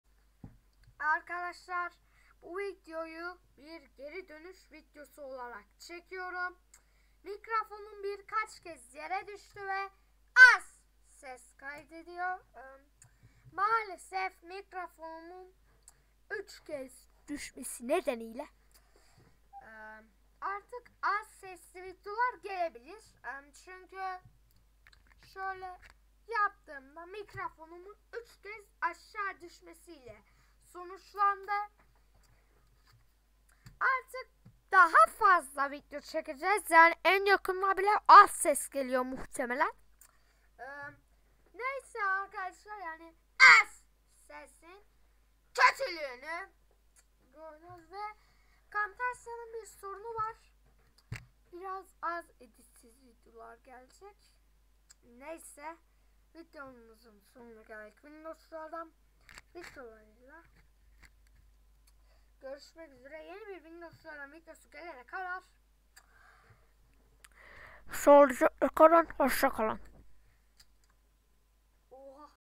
Geri dönüş videosu
Geri döndüm! Artık videolar yeniden gelecek.